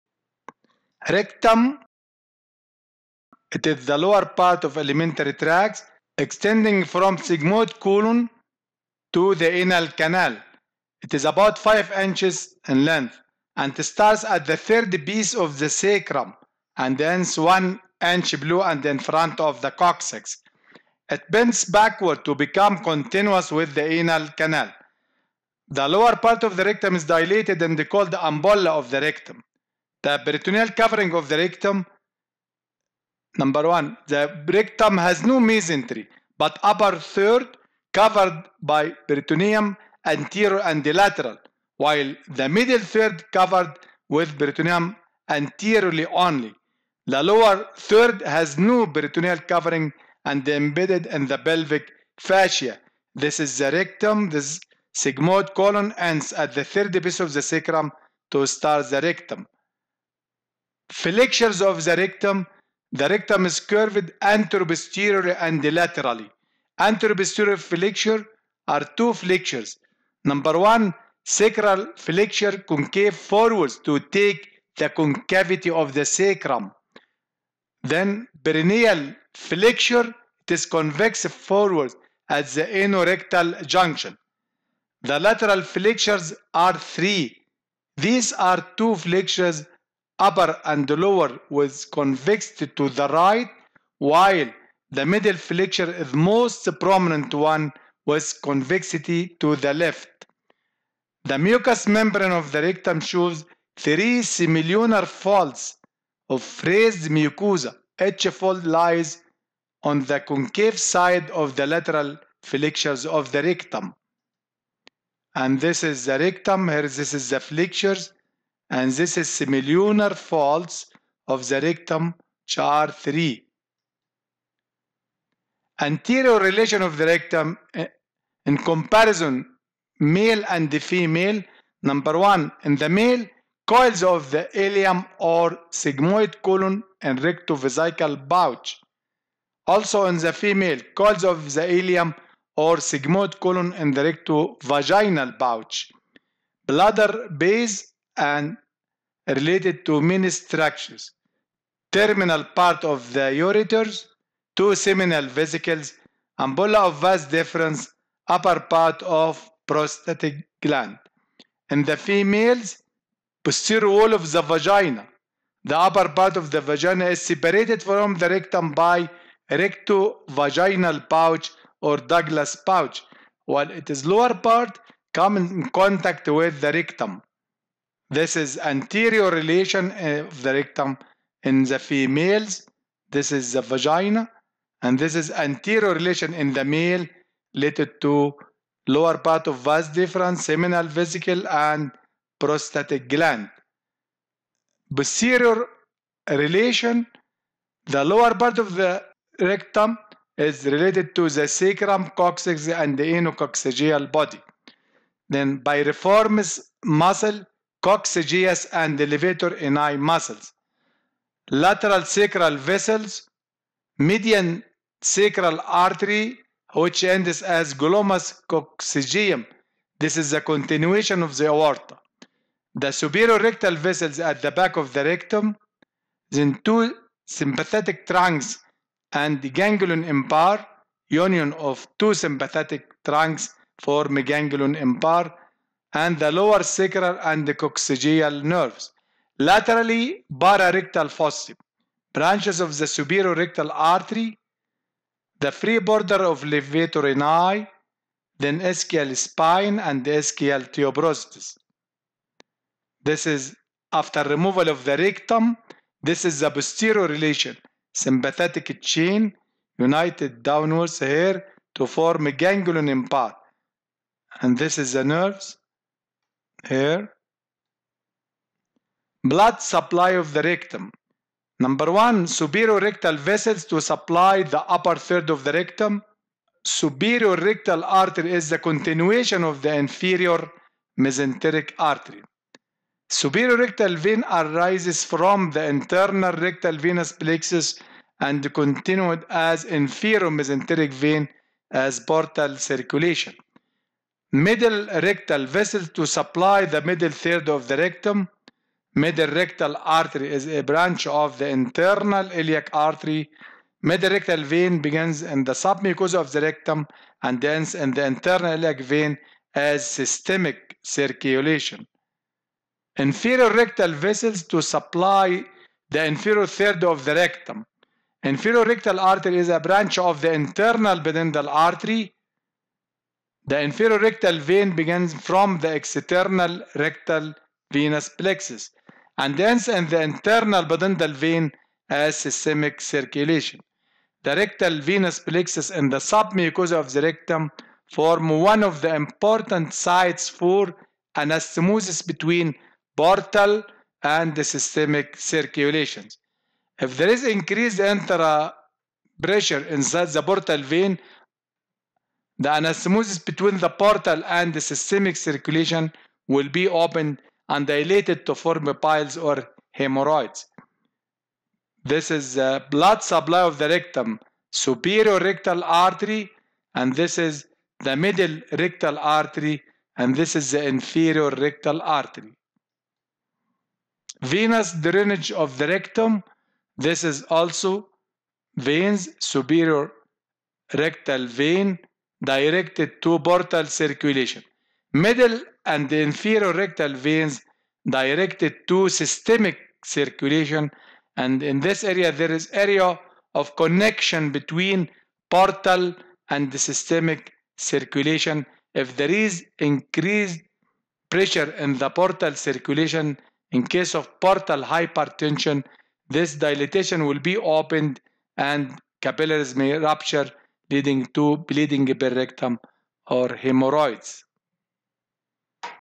Anatomy 1 | C3 - L4 | Rectum
Unit 3 : Pelvis
Lecture 4 : Rectum
1- Position of the rectum
2- Flexures of therectum
3- Blood supply of the rectum